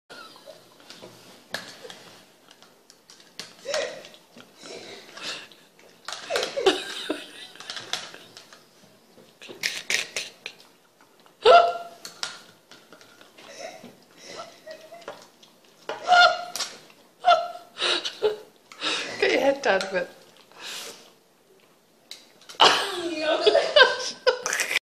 Archiebold the cockerspaniel really likes his yogurt
This is only being uploaded so that our family can see how well mannered Archie has become at the dinner table.
For all concerned it is an Activia yogurt he is devouring and a University of Bristol jumper that he is showcasing.
x

(excuse our giggling)